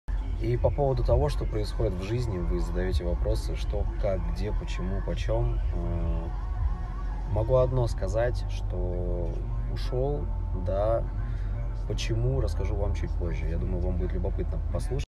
Звезда «Дома-2» Захар Саленко покинул телепроект
Возлюбленная участника реалити Яна Шафеева осталась на шоу.